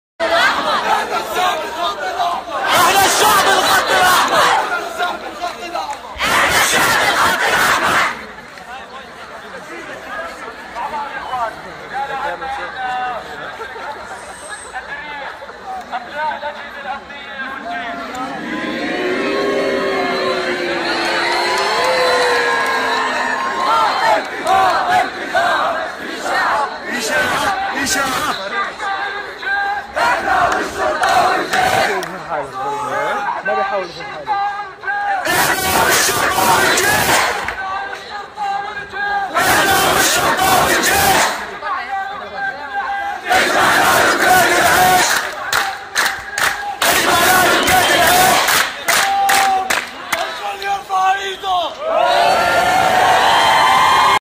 هتافات طلبة ضد قرار “الاردنية”
تلفزيون البلد - نفى طلبة الجامعة الأردنية المعتصمون ضد قرار رفع الرسوم أن يكونوا قد هتفوا بهتافات ضد الجيش العربي الأردني.
ونشر طلبة في الجامعة الأردنية جانبا من إحدى الاعتصامات في الجامعة التي تم الهتاف بها بصوت موحد “احنا والشرطة والجيش تجمعنا لقمة العيش”.
وكانت صفحات على مواقع التواصل الاجتماعي، اتهمت طلبة الجامعة الأردنية المعتصمين بالإساءة إلى الجيش، كما اتهم كُتاب في صحف يومية، المطالبة الطلابية بأنها تتبع أجندات خارجية.